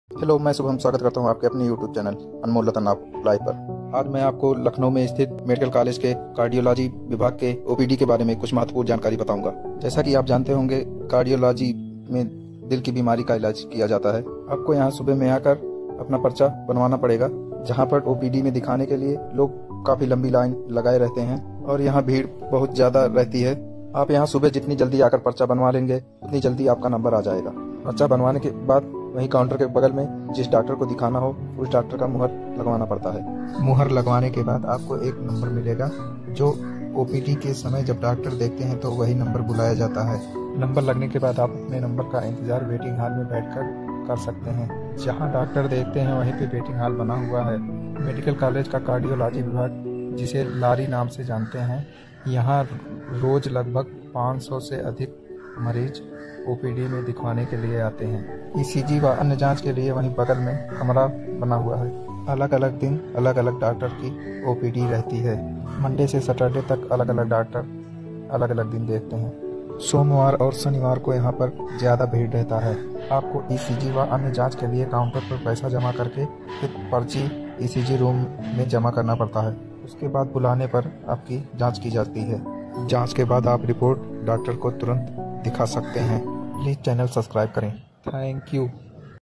Lucknow medical College "LARI" CARDIOLOGY (कार्डियोलॉजी विभाग "लाॅरी" लखनऊ मेडिकल कॉलेज)
लाॅरी मेडिकल कॉलेज लखनऊ में हार्ट समस्या (हृदय बिमारी) की ओपीडी के बारे में महत्वपूर्ण जानकारी जो आपके एवं आपके शुभचिंतकों के लिए उपयोगी हो सकती है...

location
Lari Cardiology Centre